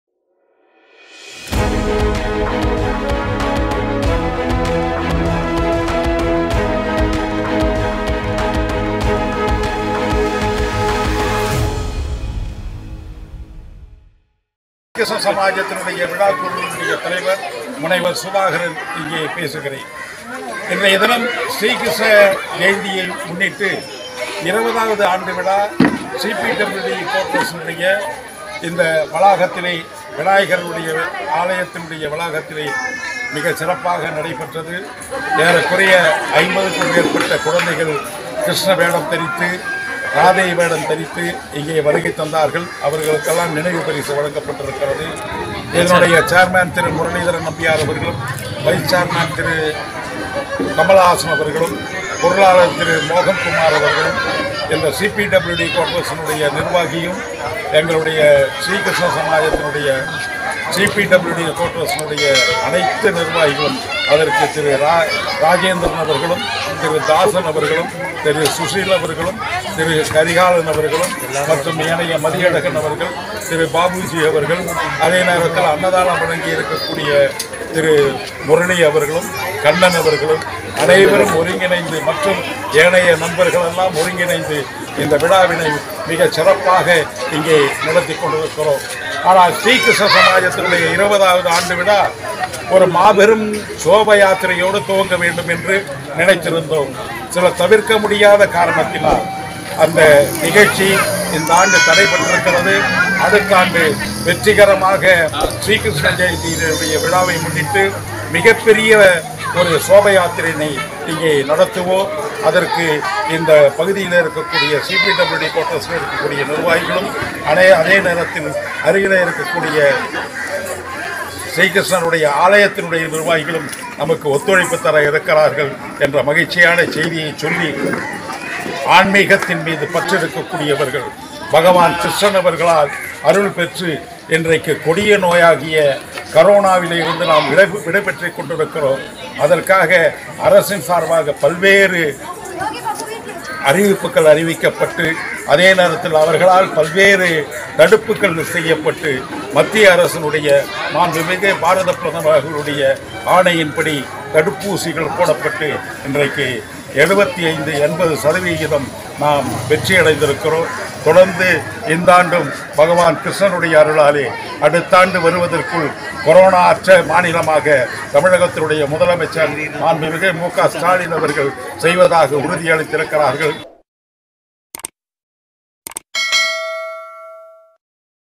ஸ்ரீ கிருஷ்ண சமாஜ்சார்பில்20ம்ஆண்டுகிருஷ்ணஜெயந்திவிழா k.k நகர் CP WDகுடியிருப்புவளாகத்தில்நடைபெற்றது
ஸ்ரீ கிருஷ்ண சமாஜ் சார்பில் 20ம் ஆண்டு கிருஷ்ண ஜெயந்தி விழா சென்னை கே கே நகர் C P W D குடியிருப்பு வளாகத்தில் நடைபெற்றதுcrime muslim varanavasi tamilnadu political newsa @jpNews @newsliv